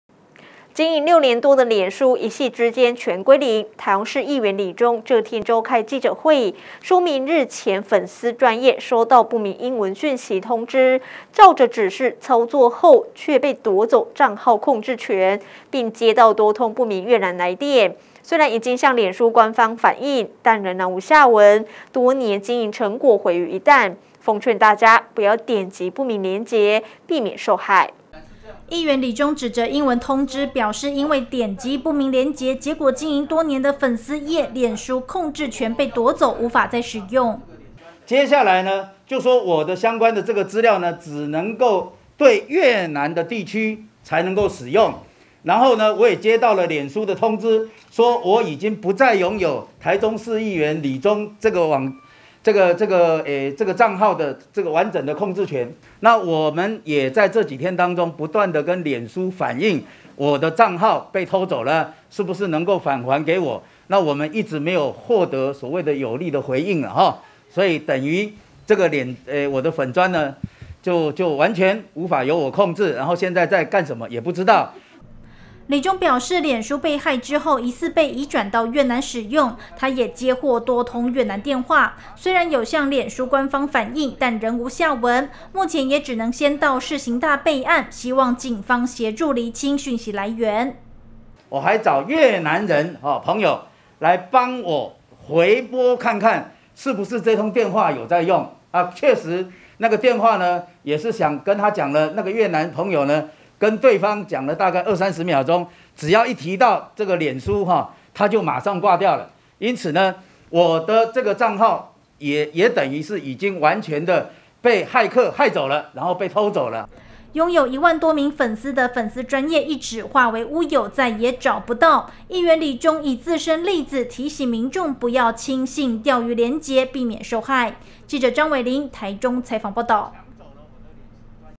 六年經營一指歸零 議員李中臉書疑被駭-大台中新聞
經營六年多的臉書，一夕之間全歸零？台中市議員李中這天召開記者會，說明日前粉絲專頁收到不明英文訊息通知，照著指示操作後，卻被奪走帳號控制權，並接到多通不明越南來電，雖然已經向臉書官方反應，但仍無下文，多年經營成果毀於一旦，奉勸大家不要點擊不明連結，免得受害。「以上新聞可從哈TV APP收視」